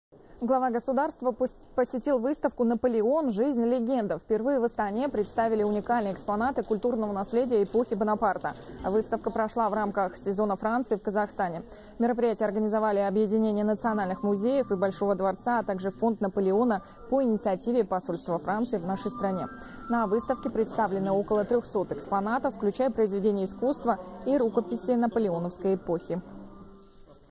Глава государства посетил выставку «Наполеон. Жизнь. Легенда»
Впервые в Астане представили уникальные экспонаты культурного наследия эпохи Бонапарта.